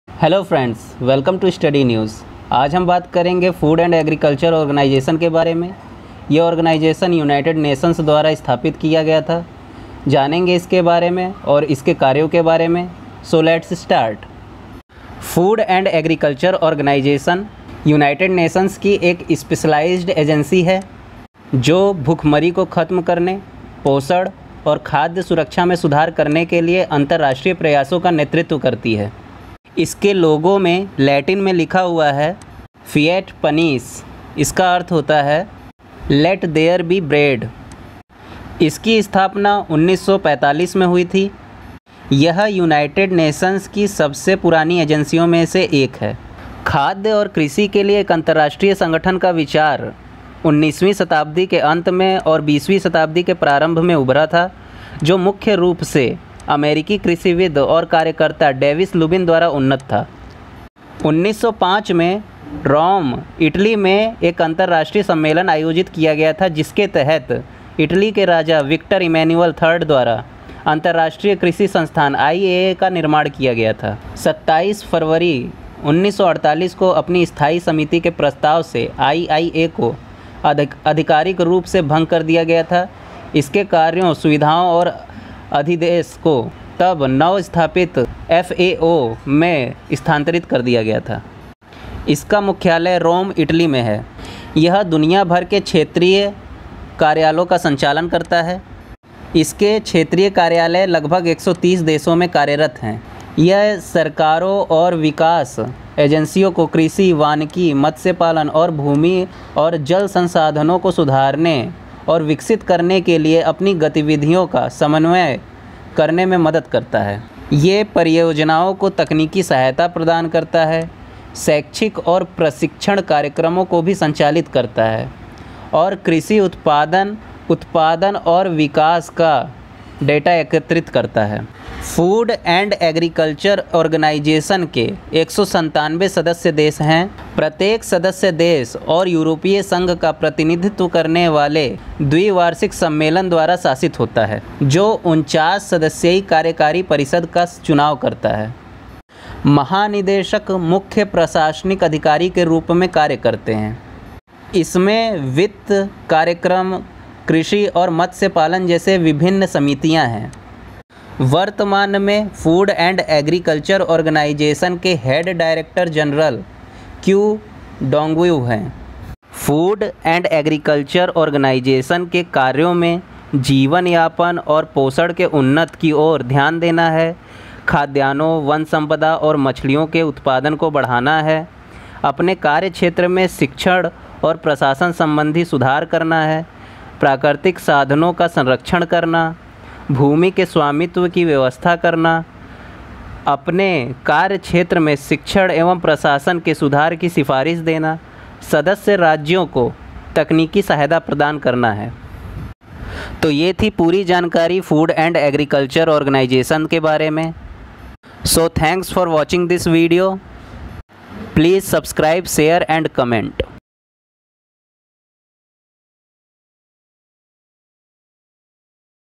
food and agriculture organisation.खाद्य और कृषि संगठन।
The Food and Agriculture Organization of the United Nations (FAO) is a specialized agency of the United Nations that leads international efforts to defeat hunger and improve nutrition and food security. Its Latin motto, fiat panis, translates to "let there be bread". Founded in October 1945, the FAO is the oldest existing agency of the U.N.
The idea of an international organization for food and agriculture emerged in the late 19th and early 20th century, advanced primarily by Polish-born American agriculturalist and activist David Lubin. In May–June 1905, an international conference was held in Rome, Italy, which led to the creation of the International Institute of Agriculture (IIA) by the King of Italy, Victor Emmanuel III
The IIA was officially dissolved by resolution of its Permanent Committee on 27 February 1948. Its functions, facilities, and mandate were then transferred to the newly established FAO, which maintained its headquarters in Rome.
The FAO is headquartered in Rome, Italy and maintains regional and field offices around the world, operating in over 130 countries. It helps governments and development agencies coordinate their activities to improve and develop agriculture, forestry, fisheries, and land and water resources. It also conducts research, provides technical assistance to projects, operates educational and training programs, and collects data on agricultural output, production, and development.
Composed of 197 member states, the FAO is governed by a biennial conference representing each member country and the European Union, which elects a 49-member executive council. The Director-General, currently Qu Dongyu of China, serves as the chief administrative officer. There are various committees governing matters such as finance, programs, agriculture, and fisheries